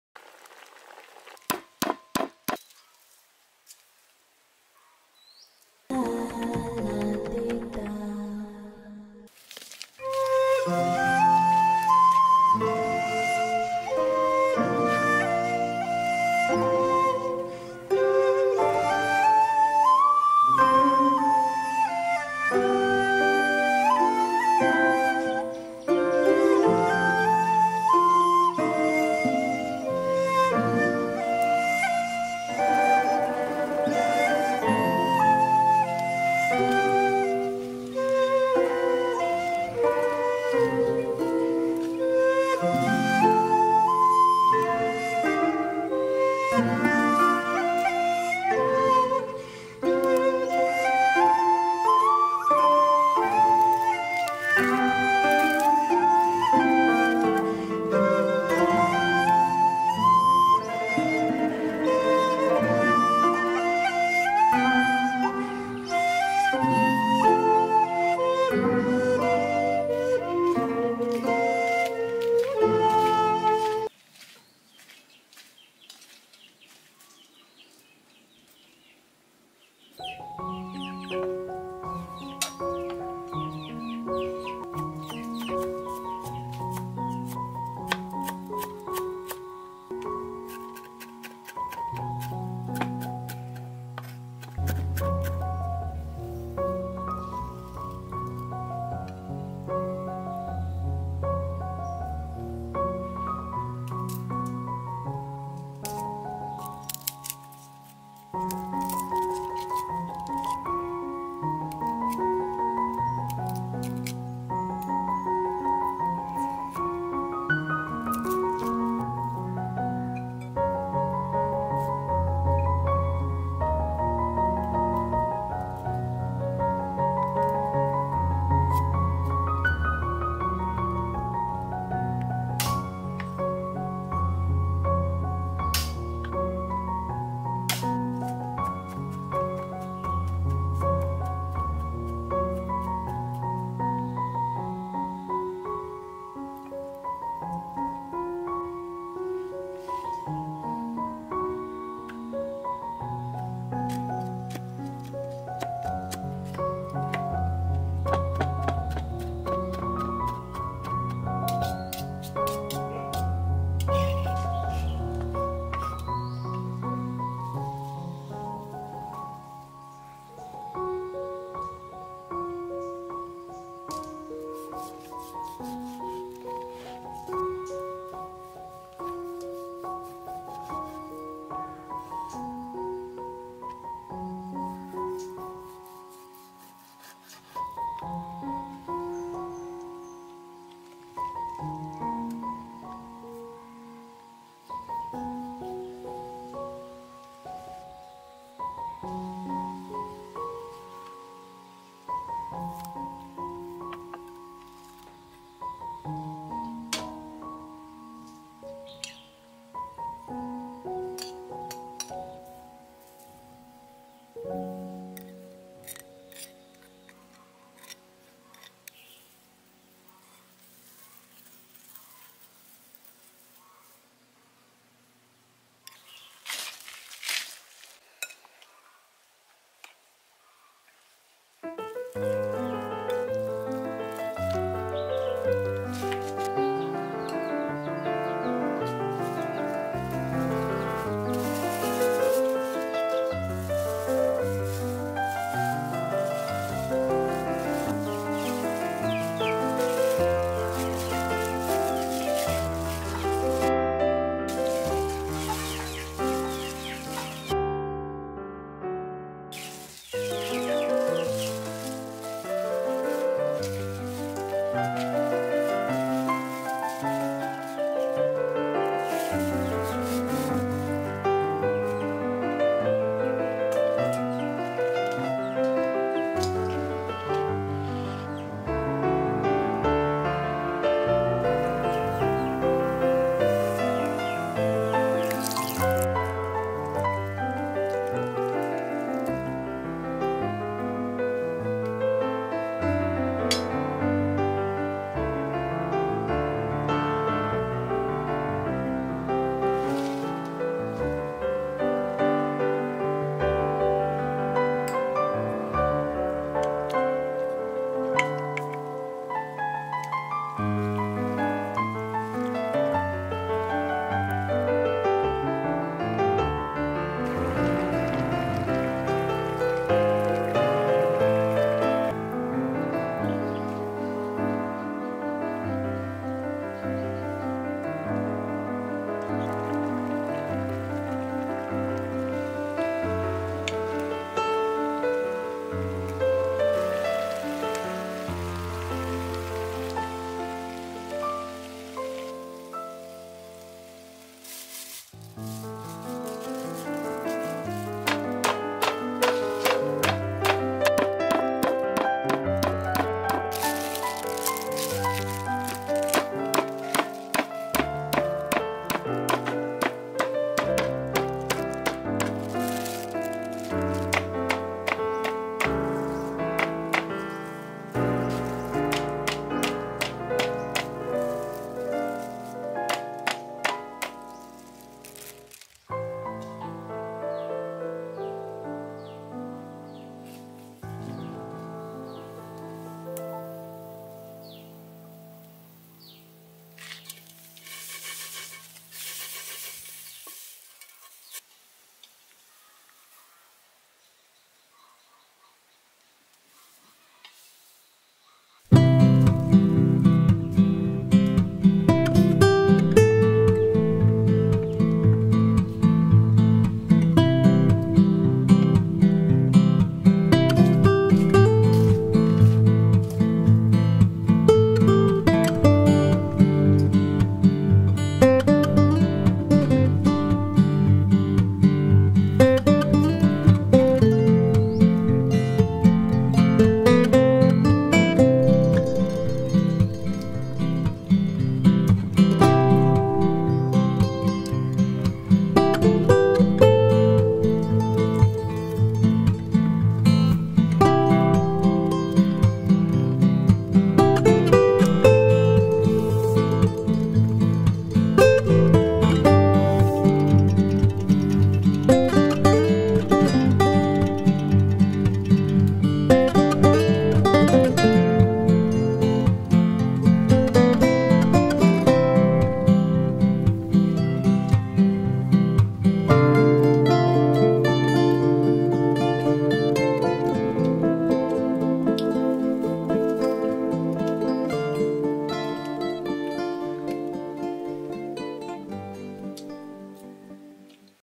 Hong made the sauté super spicy beef jerky served with lime
❤️Welcome to Hong Daily Life #40❤️